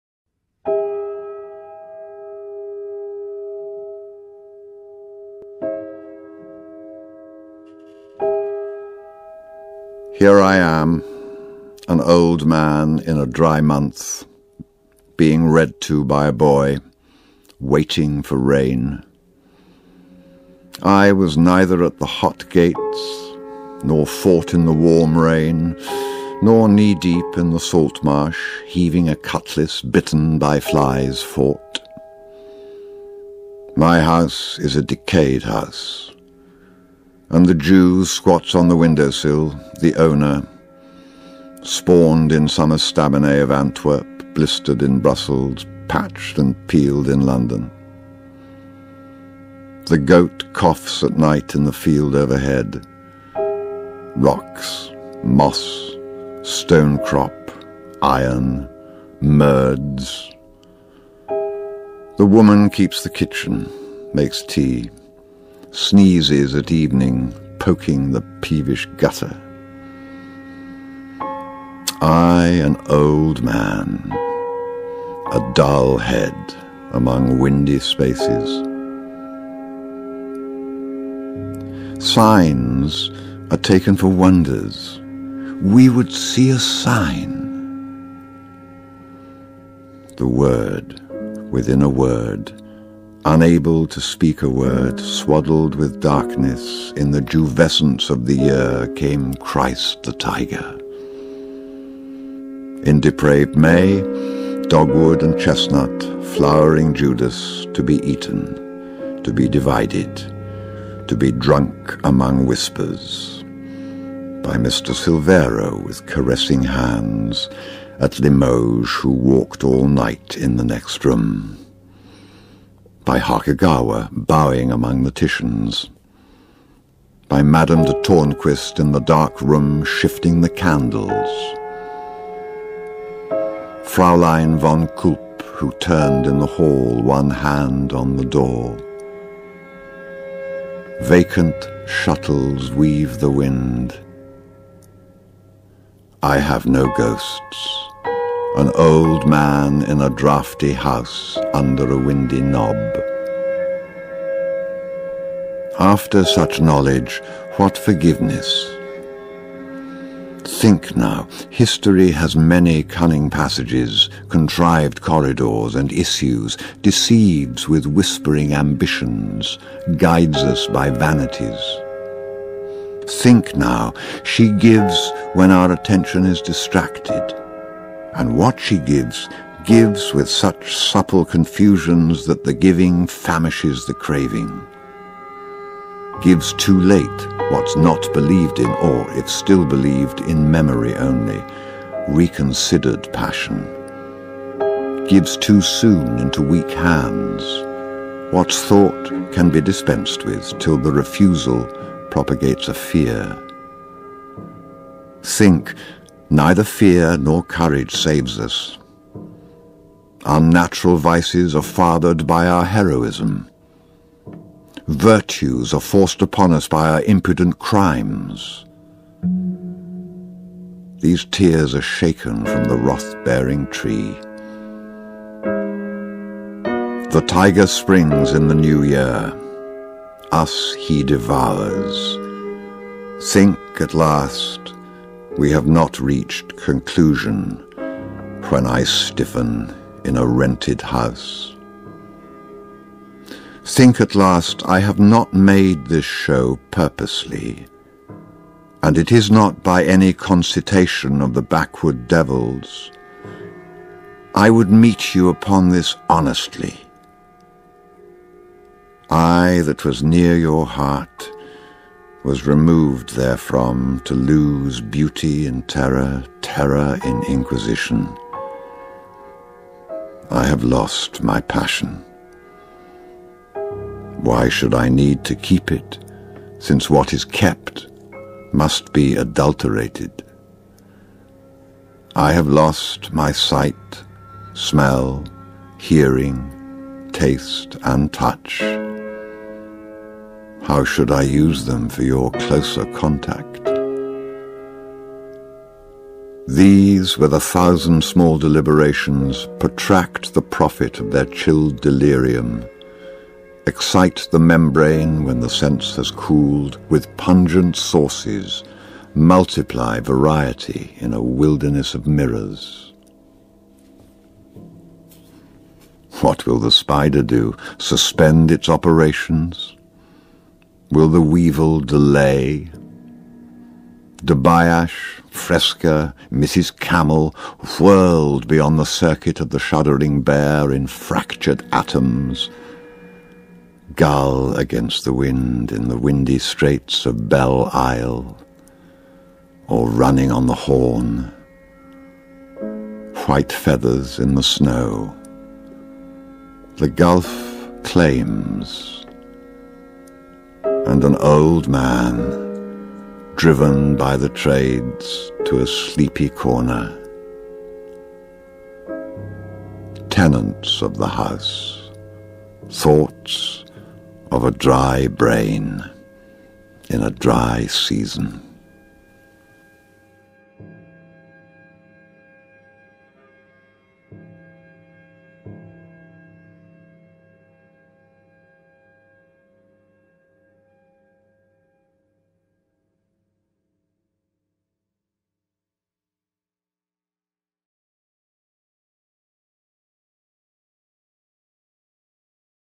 "Gerontion" by T.S Eliot |read by Jeremy Irons
Jeremy Irons recorded for BBC Radio 4 five episodes of reading from T.S Eliot's work, all available
The poem is titled "Gerontion" and is part of Episode 2.